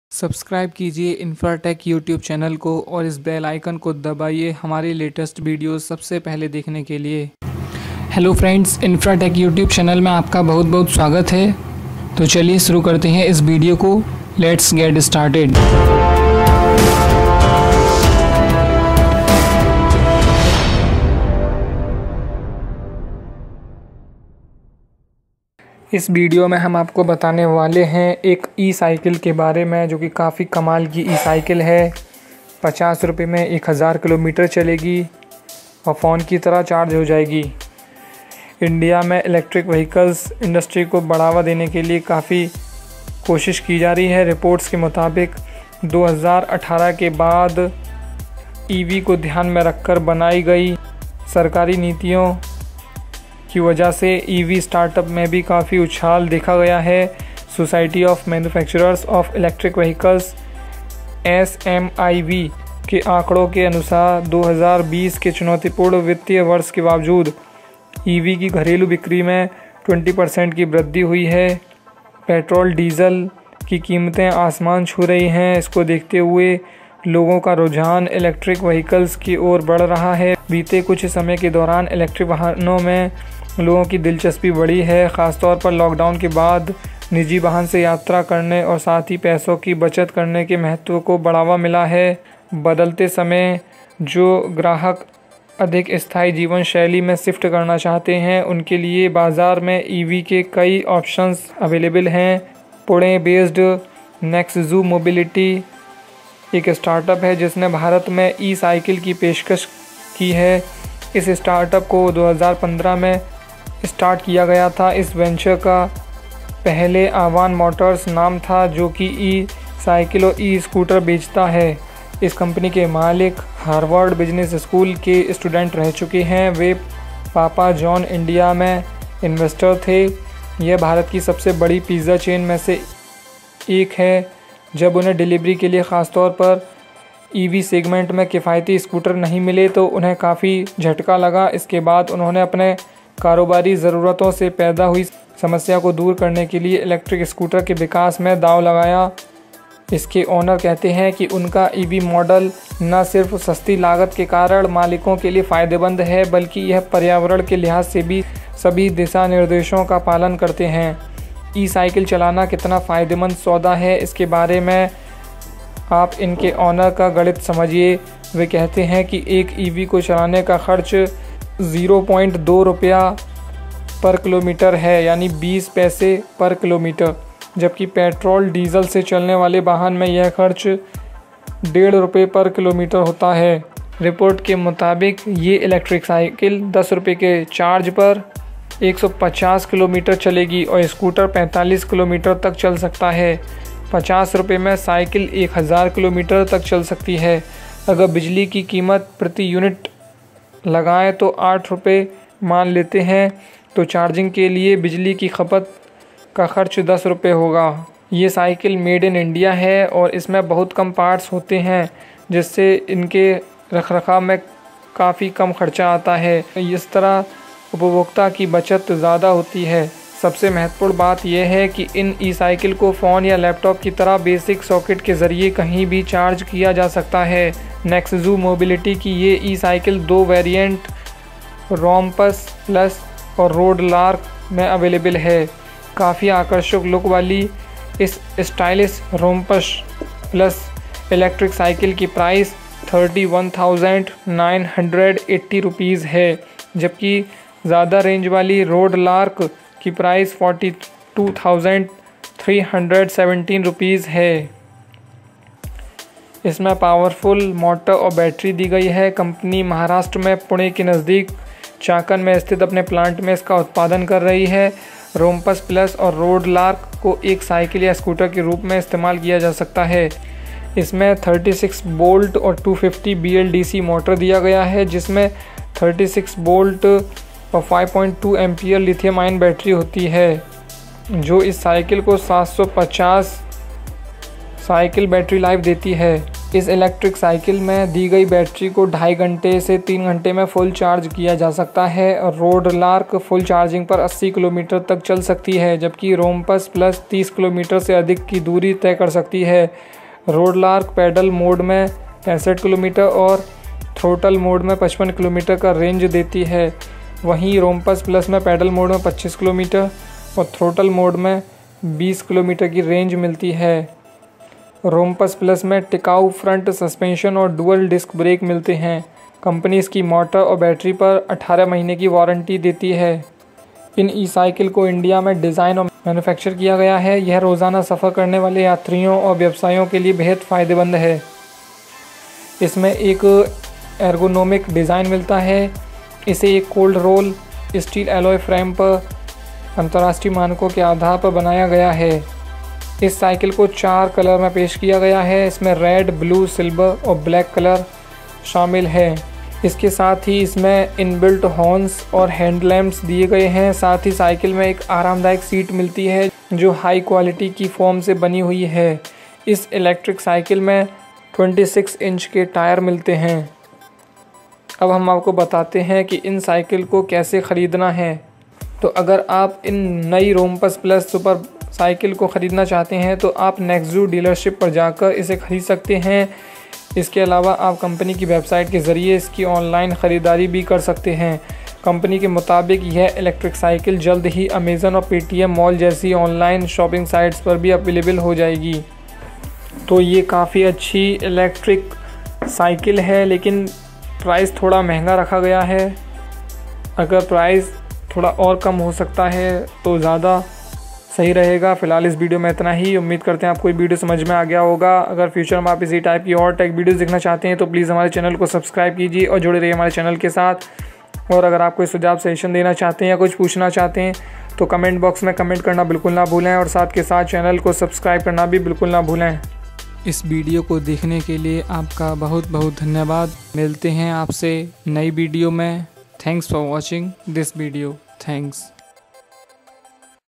NEXZU MOBILITY launched 2 latest electric cycles Price, Review and Specifications | Infra Tech
In this video I make a review of NEXZU MOBILITY launched 2 latest electric cycles Price, Review and Specifications | Infra Tech.